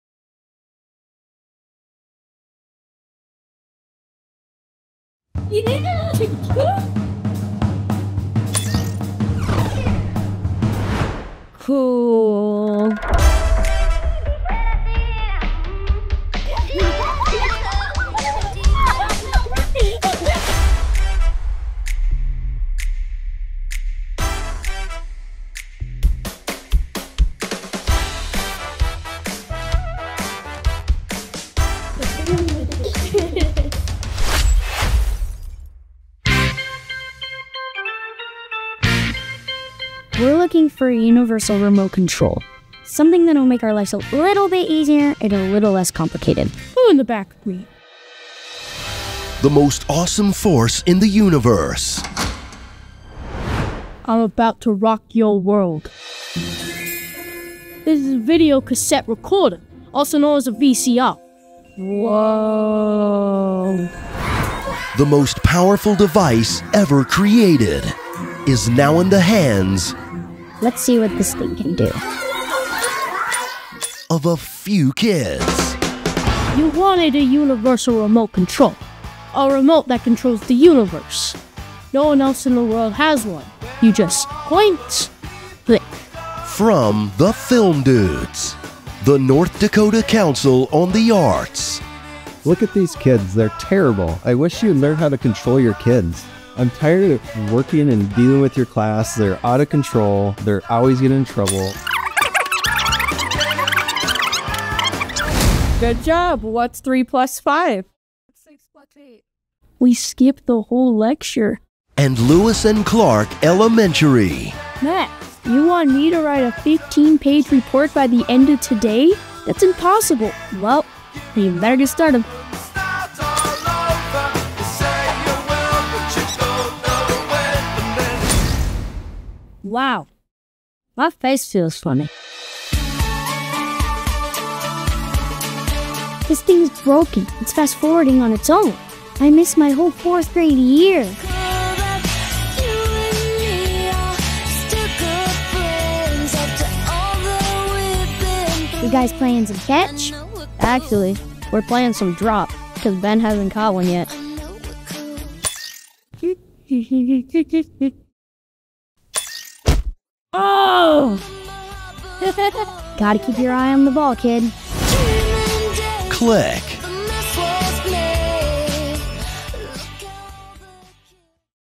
Click - Theatrical Trailer (2024) PARODY
Imagine, the world's most powerful device...now in the hands of fourth graders. However, what would it cost them to use such a device?

Working with Lewis & Clark Elementary School, we created a trailer inspired by the film "Click."

All of this is made possible by Lewis & Clark Elementary School and the North Dakota Council on the Arts.